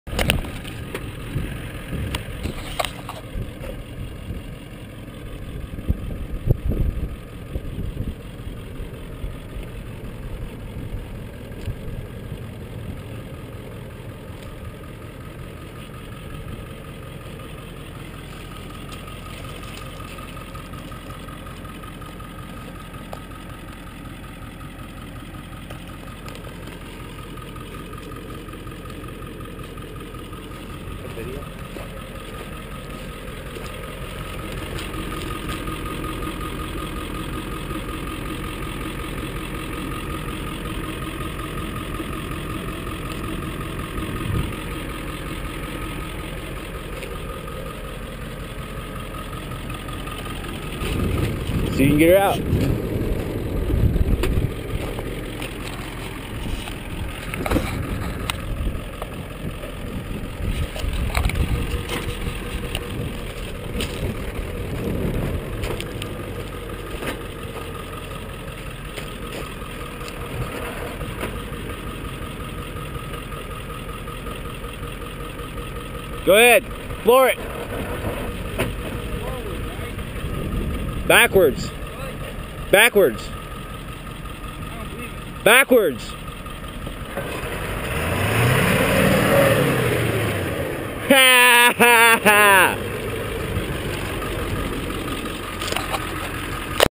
Long Wheelbase 4x4 Test
putting through the desserts of Quartzsite, AZ, I didn't see this wash, and had no desire to learn how she'd do backing out of this mishap, and it looked to me like forward wasn't going to help